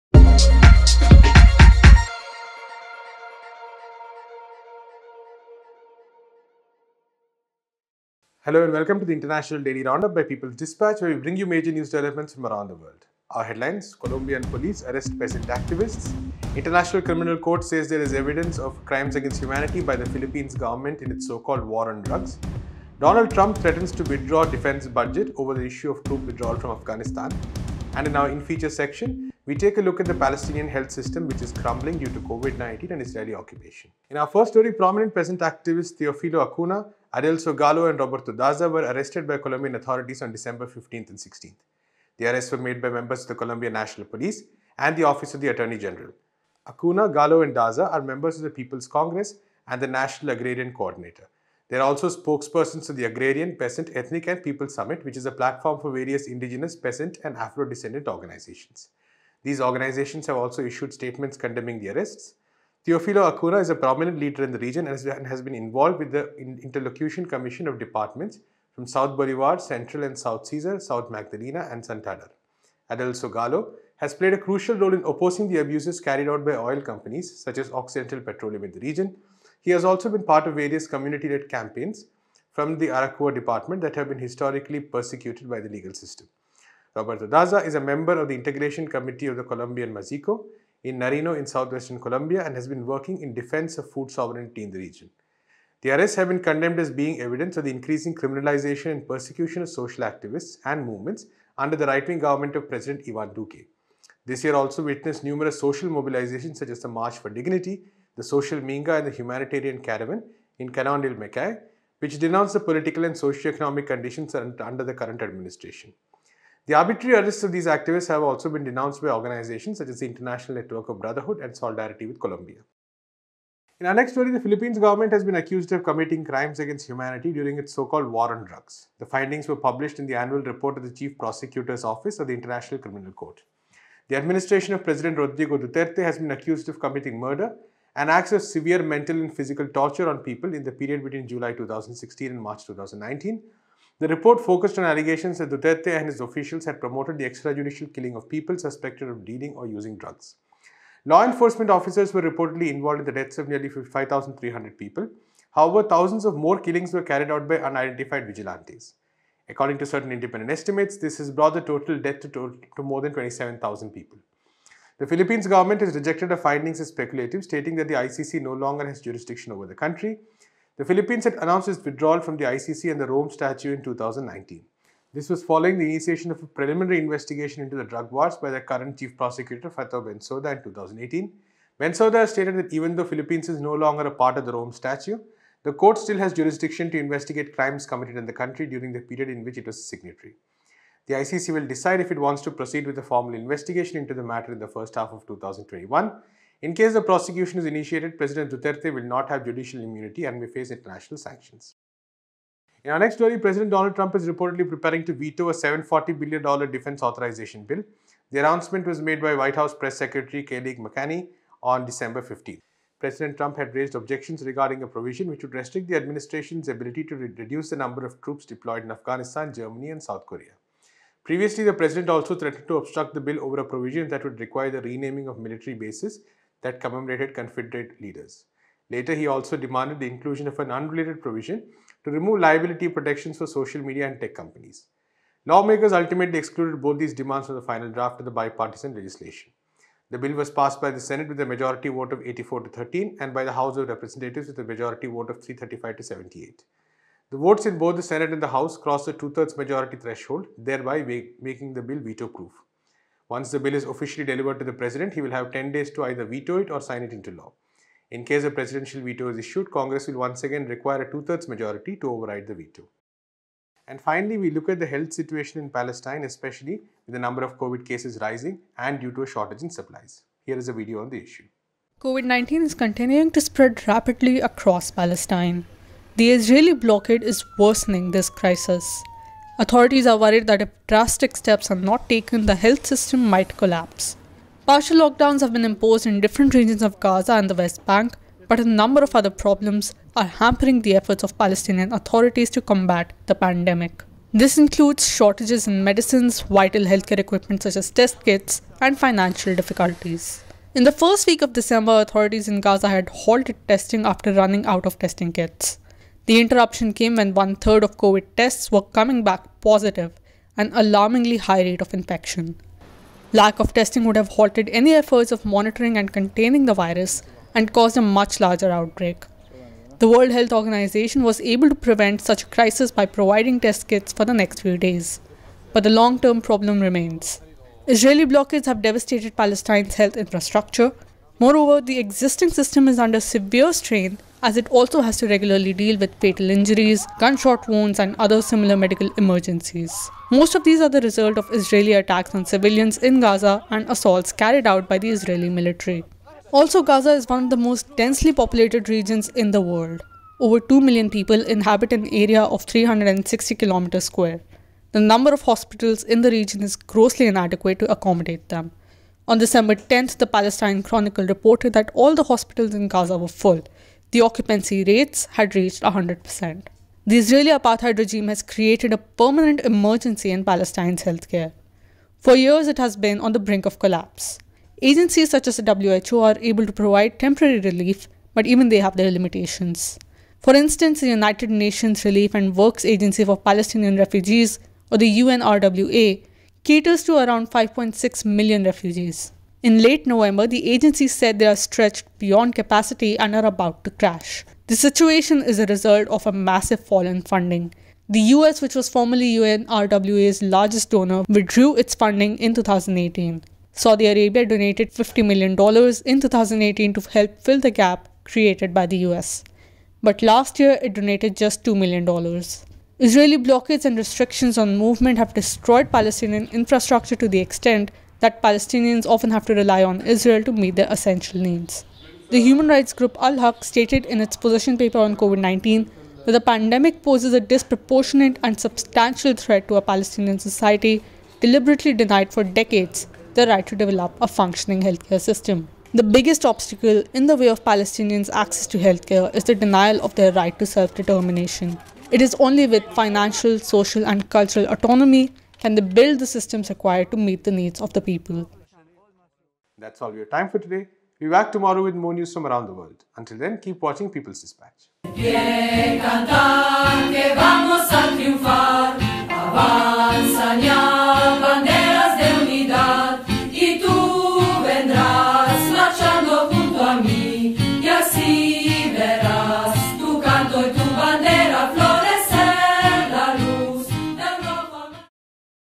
ICC Report finds ‘reasonable basis’ for evidence of crimes against humanity in Philippines
Today's headlines-
1.Colombian National Police arrests prominent peasant leaders
2. Philippines government  accused of crimes against humanity during anti-drug campaign
3. Trump threatens to veto defense bill over troop withdrawals from Afghanistan
4. Palestinian health system crumbles due to COVID-19, Israelli occupation